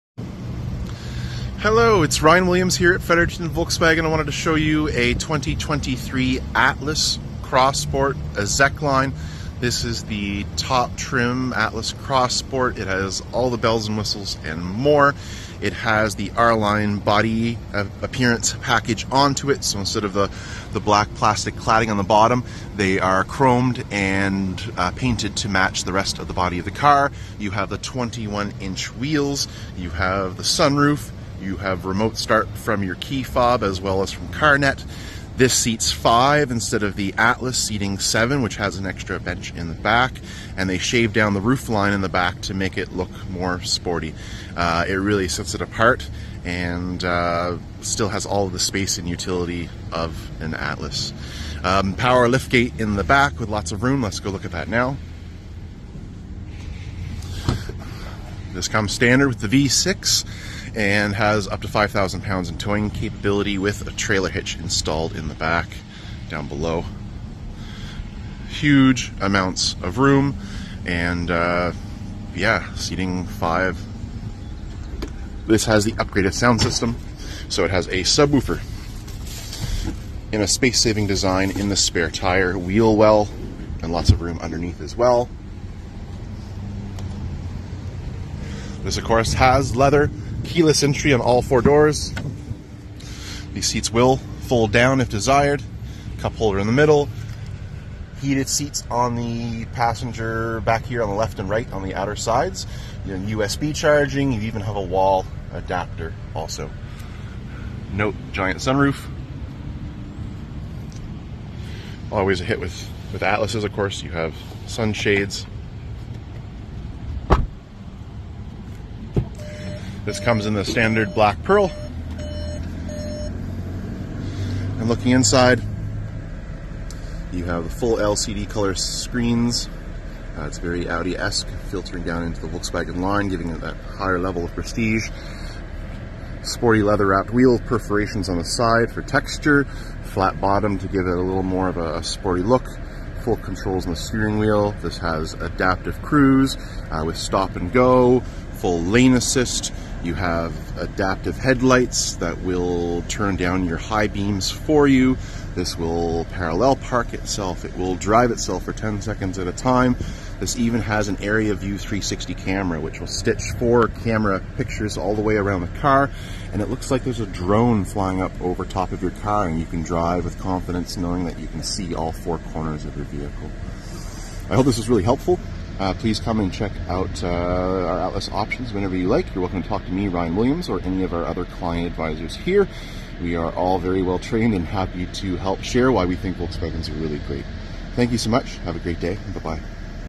2023 Cross Sport
Stock #23-174 OR Stock # 23-157.  Execline R-Line in Deep Black Pearl with Titan Black Interior